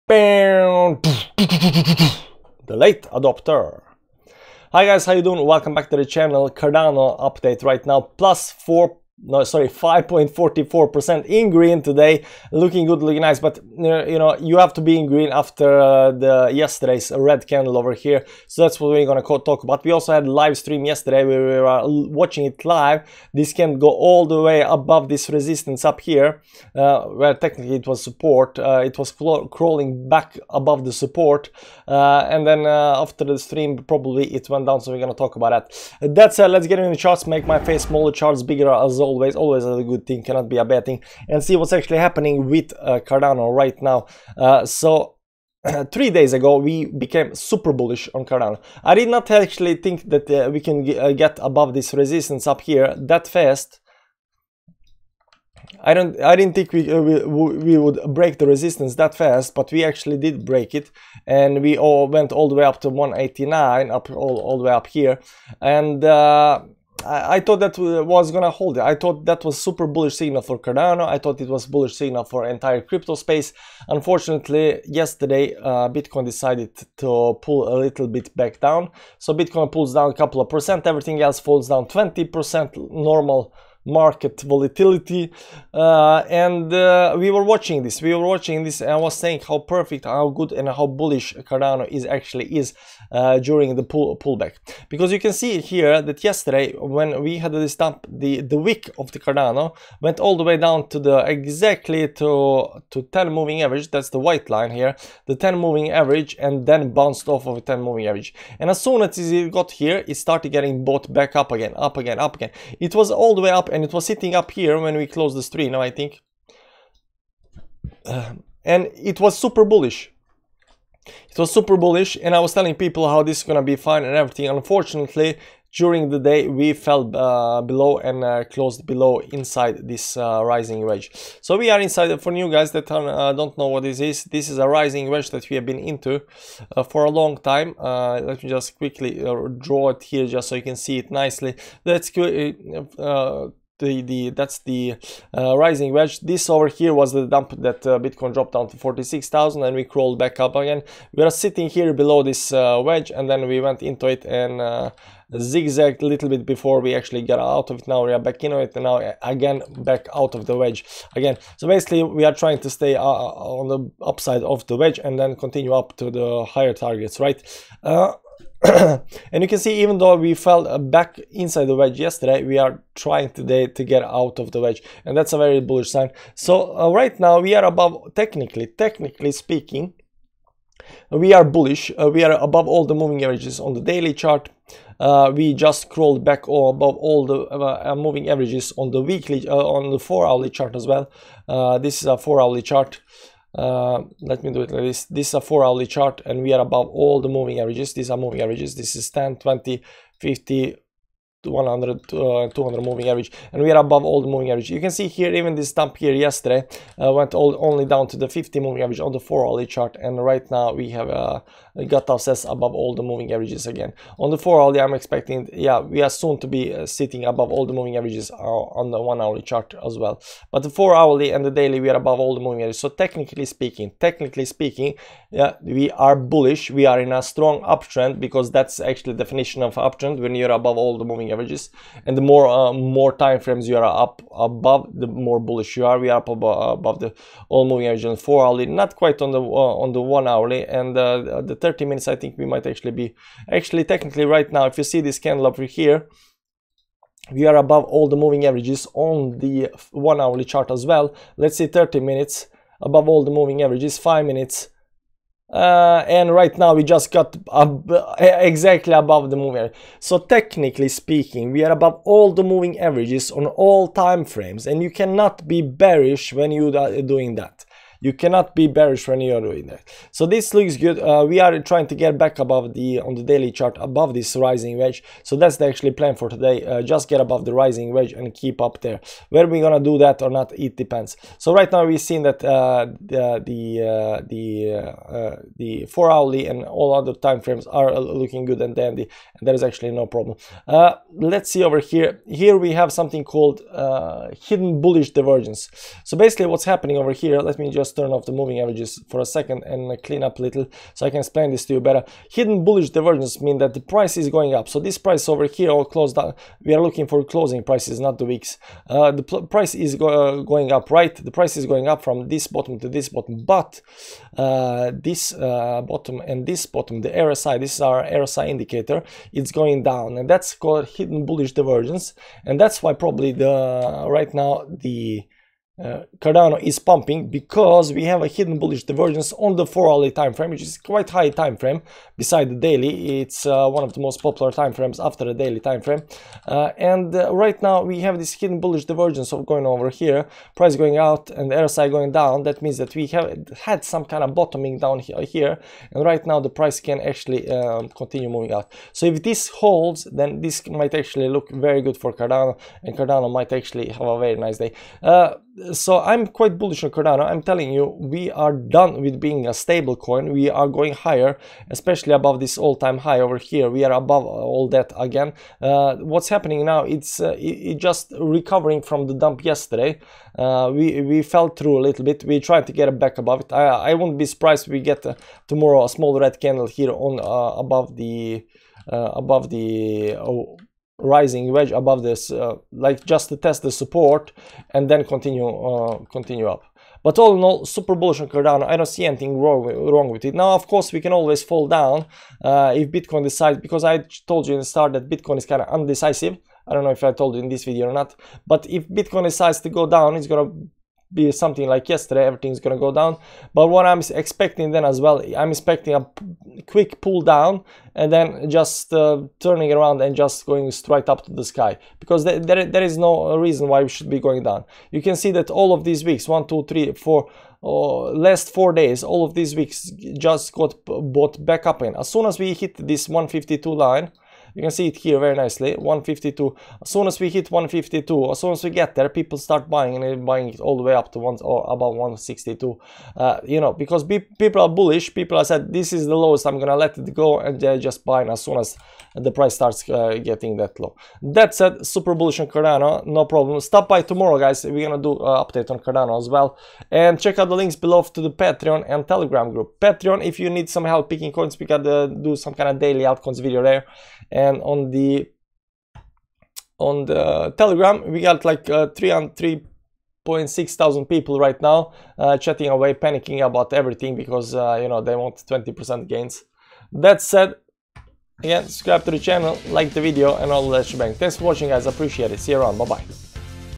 Going Back Above The Wedge - Cardano ADA Analysis And Price Prediction.
In today's crypto video, we are doing Cardano ADA technical analysis and we will try to figure out next move for Cardano ADA price!

⭐ Binance (-10% on fees when trading)

________________________________ THE END